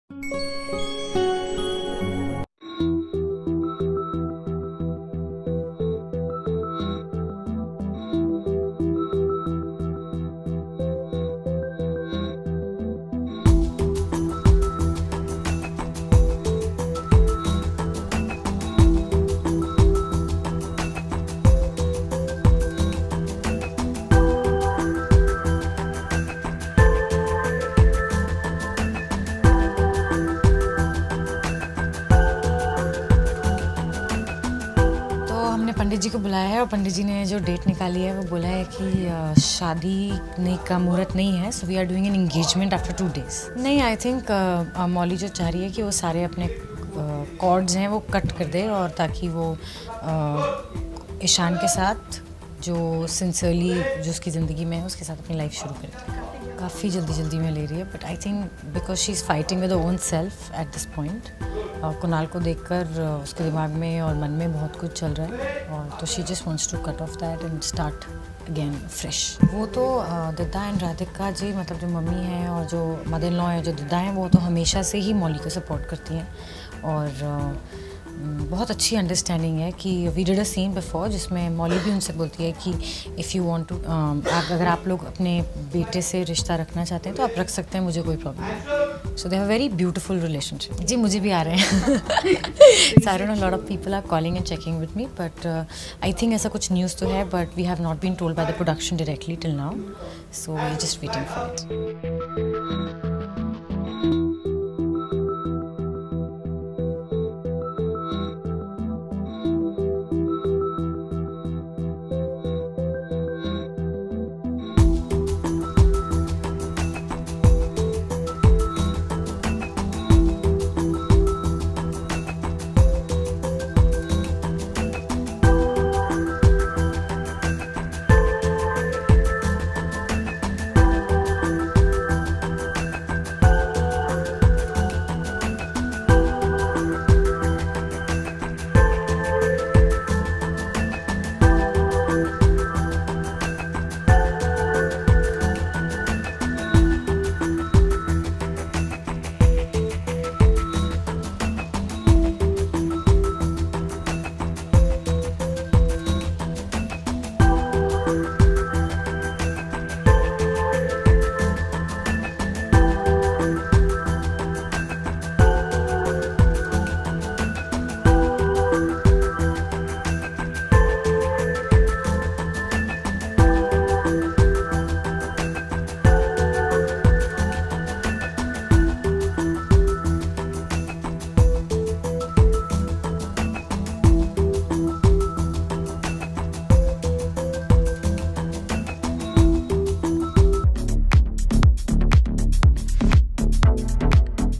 Silsila Badalte Rishton Ka: Mauli & Ishaan's Engagement Preparation l Aditi Sharma IV
Silsila Badalte Rishton Ka: Mauli & Ishaan's Engagement Preparation, Kunal In Shock l Aditi Sharma IV
OnLocation